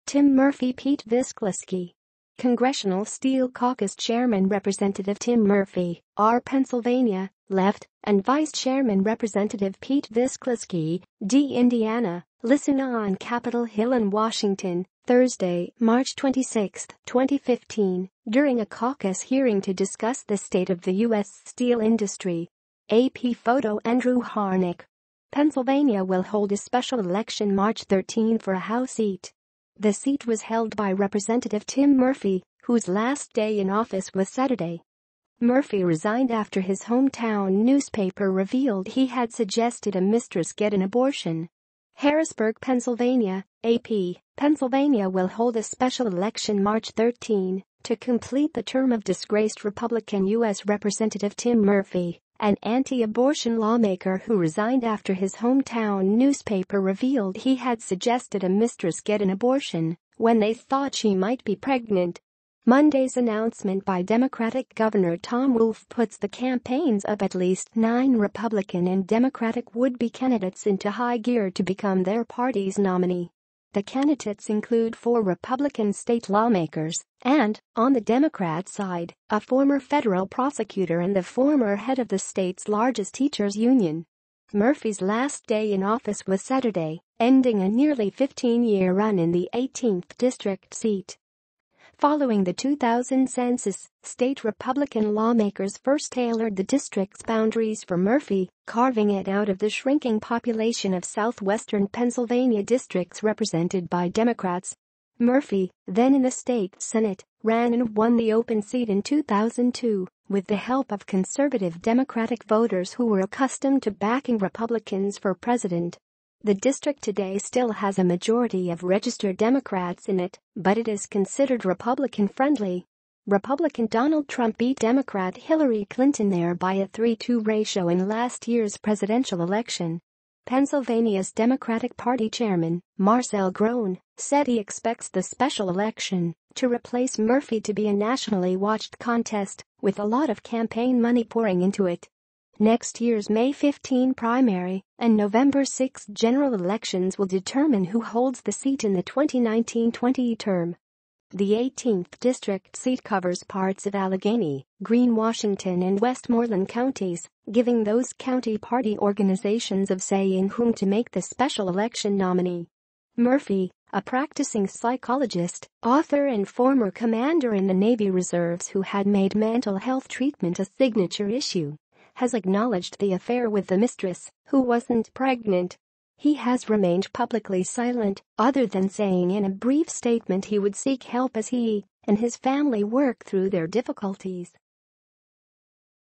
Pennsylvania governor sets special election to fill the seat of a GOP congressman who resigned amid
Tim Murphy Pete Visclosky
Congressional Steel Caucus Chairman Rep. Tim Murphy, R-Pa., left, and Vice Chairman Rep. Pete Visclosky, D-Ind., listen on Capitol Hill in Washington, Thursday, March 26, 2015, during a caucus hearing to discuss the state of the U.S. steel industry. AP Photo/Andrew Harnik

Pennsylvania will hold a special election March 13 for a House seat.
The seat was held by Rep. Tim Murphy, who's last day in office was Saturday.
Murphy resigned after his hometown newspaper revealed he had suggested a mistress get an abortion.
HARRISBURG, Pa. (AP) — Pennsylvania will hold a special election March 13 to complete the term of disgraced Republican U.S. Rep. Tim Murphy, an anti-abortion lawmaker who resigned after his hometown newspaper revealed he had suggested a mistress get an abortion when they thought she might be pregnant.

Monday's announcement by Democratic Gov. Tom Wolf puts the campaigns of at least nine Republican and Democratic would-be candidates into high gear to become their party's nominee. The candidates include four Republican state lawmakers and, on the Democrats side, a former federal prosecutor and the former head of the state's largest teachers union.

Murphy's last day in office was Saturday, ending a nearly 15-year run in the 18th District seat.

Following the 2000 census, state Republican lawmakers first tailored the district's boundaries for Murphy, carving it out of the shrinking population of southwestern Pennsylvania districts represented b